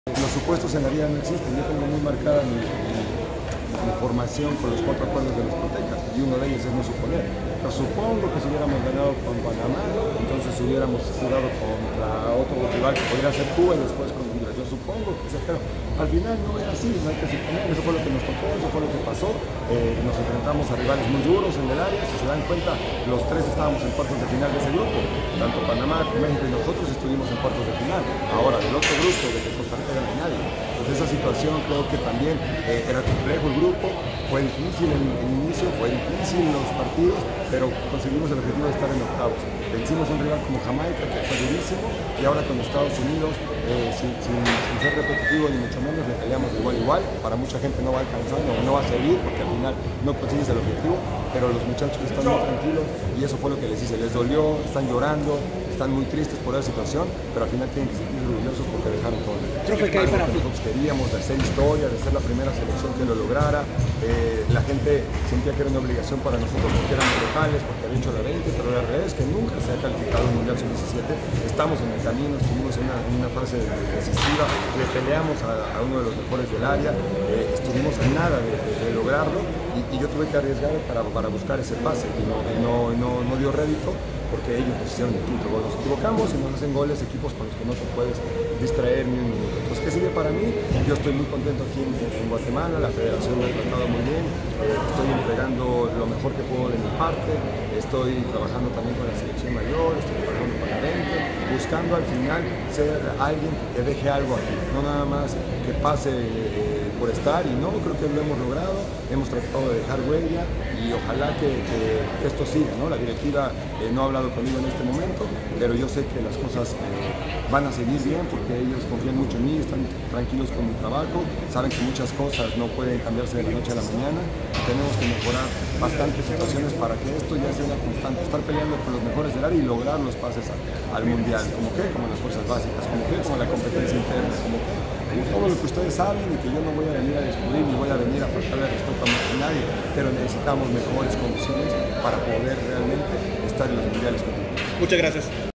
Marvin Cabrera, técnico de la sub-17, habla tras la eliminación de Guatemala en el Premundial
El estadio Doroteo Guamuch Flores vivió un encuentro no apto para cardiacos entre las selecciones Sub-17 de Guatemala y Estados Unidos, el partido de los cuartos de final del Premundial de Concacaf tenía la clasificación al Mundial de Perú 2023 en juego y fueron los norteamericanos quienes se llevaron el triunfo por 5-3.

Con este resultado se termina el sueño de Guatemala en el Premundial, torneo en el cual los errores en defensa le dejaron sin el sueño de estar en la Copa del Mundo de este año. Para Estados Unidos este triunfo les hace estar entre los cuatro mejores de la región y representar a Concacaf en la máxima cita de selecciones.

Tras la eliminación a manos de Estados Unidos, estas fueron las impresiones del técnico Marvin Cabrera: